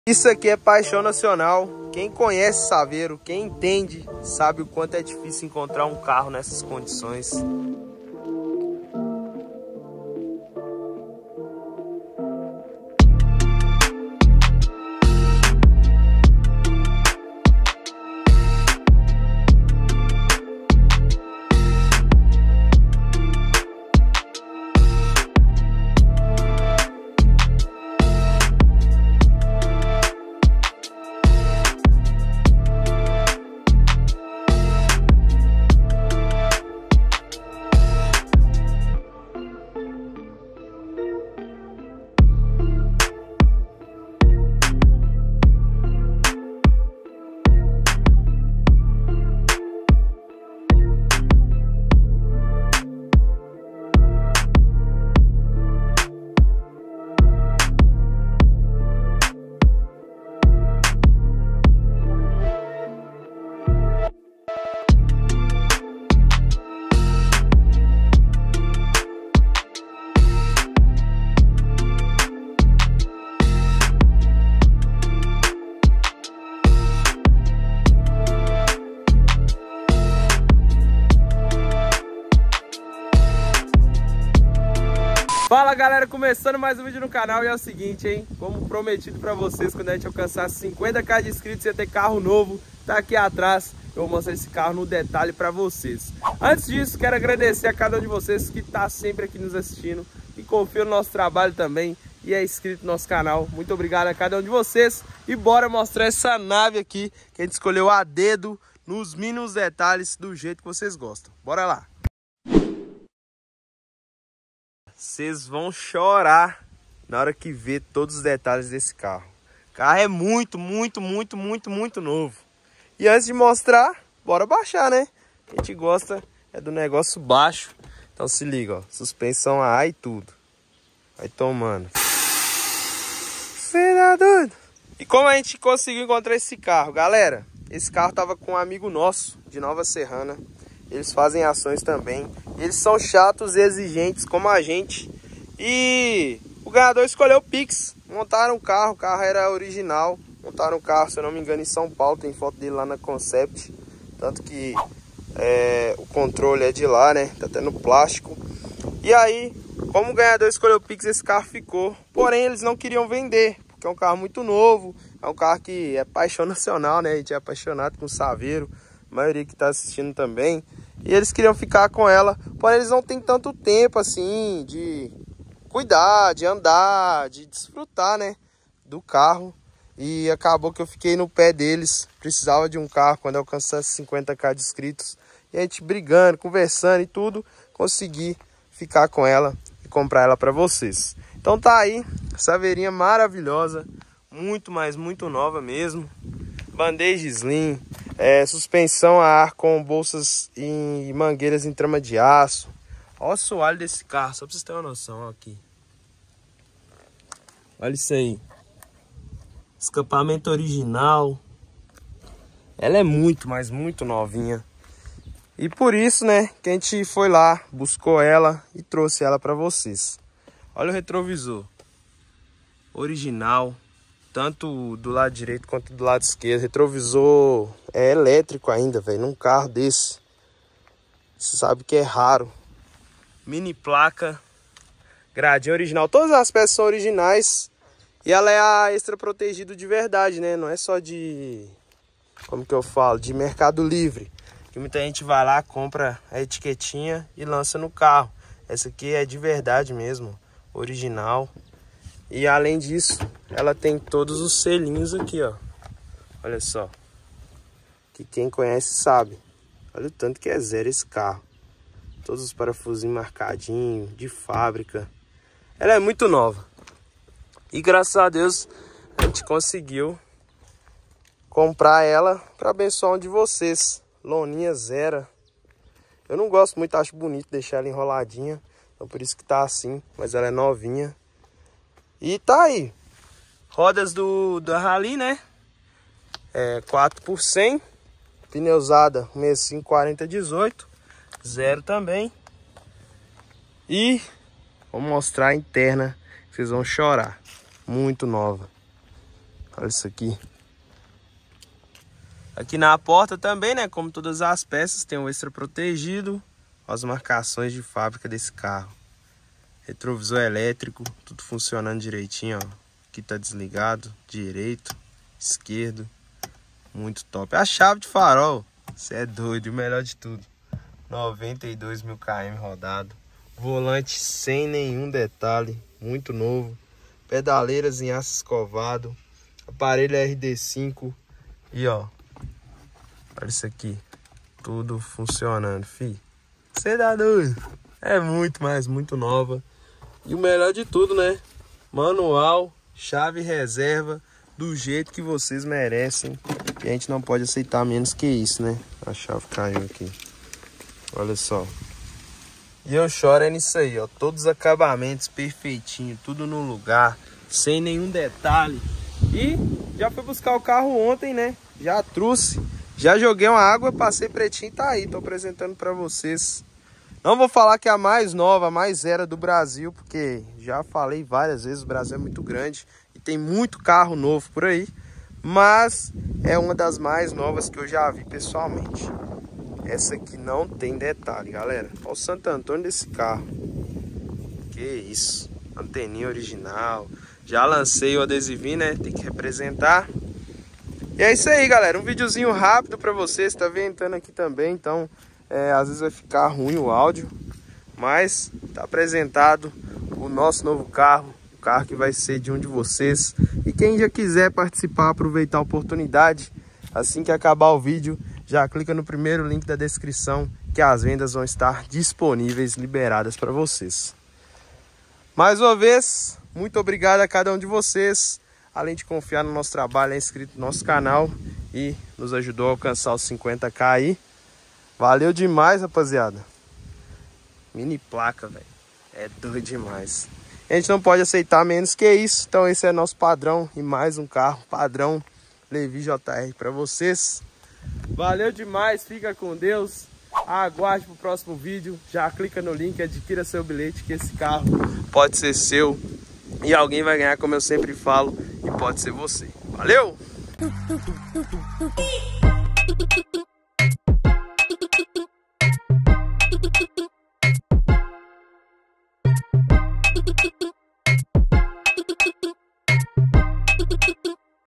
COMPREI UMA SAVEIRO SUPER SURF, PAIXÃO NACIONAL
Quem nunca foi apaixonado numa saveiro? confira o vídeo ate o final e deixe seu comentario!